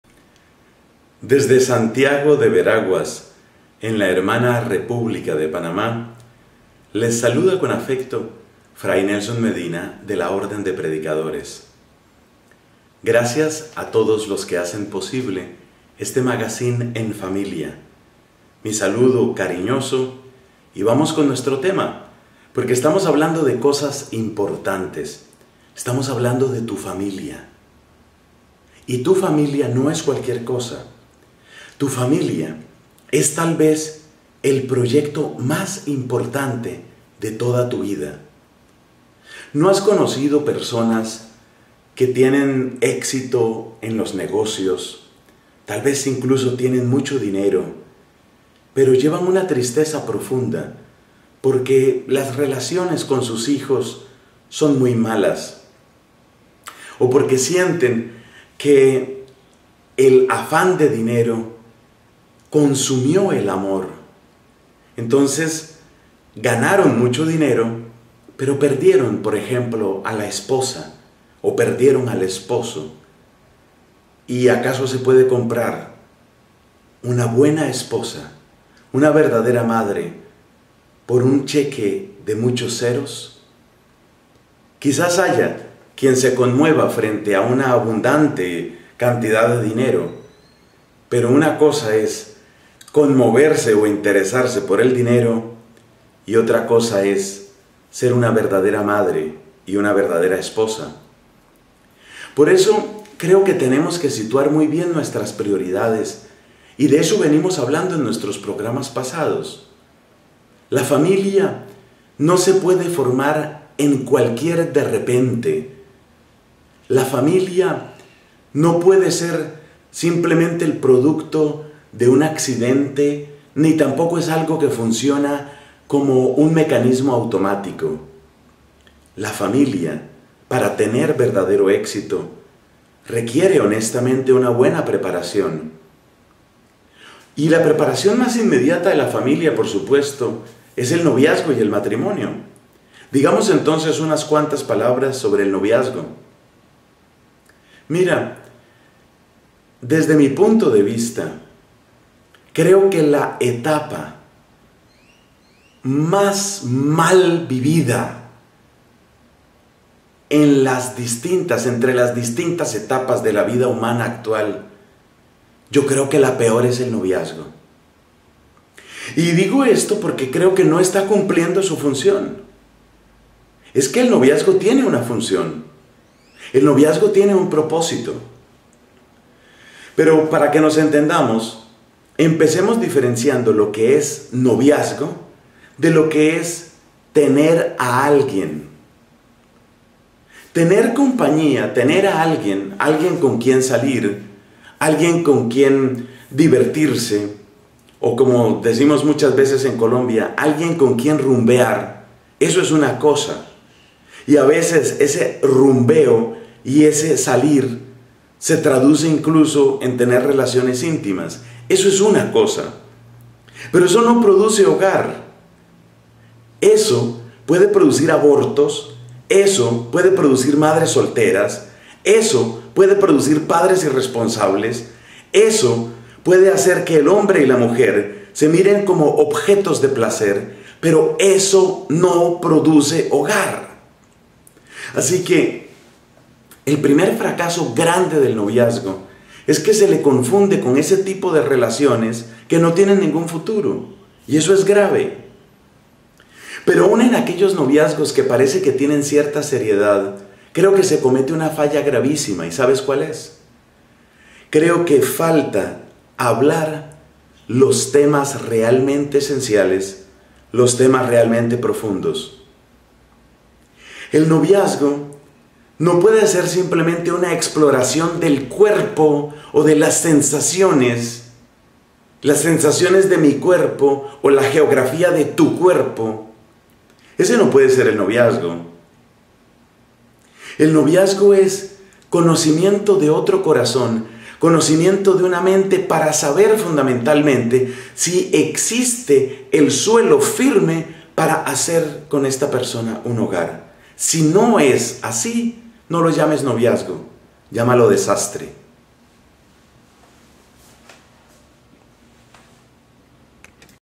ENFAMILIA 02-11, El Noviazgo
Un verdadero noviazgo es mucho más que "tener a alguien." Un verdadero noviazgo mira a la conformación de un hogar.